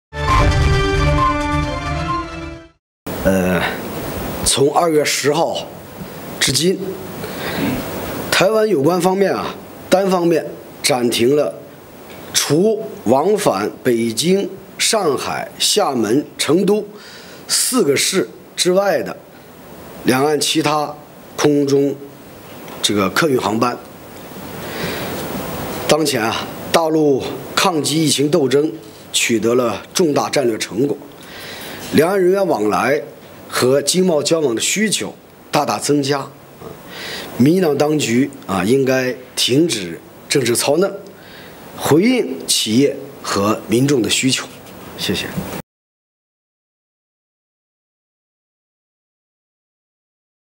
台當局取消增加大陸6個貨運航點 國台辦：民進黨當局應停止政治操弄，回應需求
國台辦發布 台交通部門近期研議增加大陸6個貨運航點，但因台陸委會的阻撓而取消。國台辦發言人馬曉光對此表示，從2月10日至今，台灣有關方面單方面暫停了除往返北京、上海（浦東及虹橋）、廈門、成都4市之外的兩岸其他空中客運航班。當前，大陸抗擊疫情鬥爭取得重大戰略成果，兩岸人員往來和經貿交往的需求大大增加。民進黨當局應停止政治操弄，回應企業和民眾的需求。